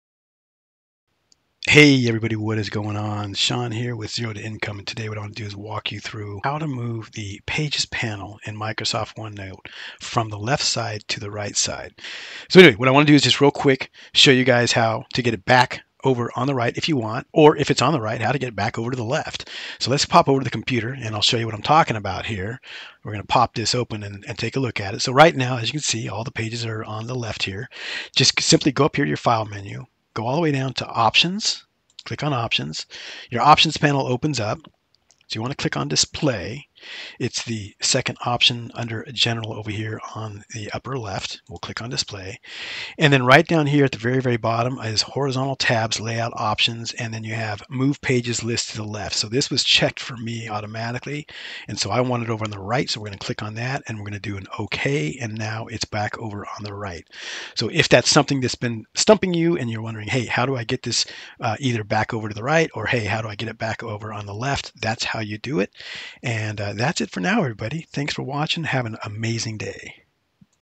How to Move Microsoft OneNote Page List to the Right - A Quick Guide
Are you looking to customize your Microsoft OneNote workspace? In this quick and easy tutorial, I'll show you how to move the Page List from the left side to the right side. If you like to have your page list on the left, follow along and make your OneNote experience more efficient!

📢 Follow Zero2Income for more tech tips and tutorials!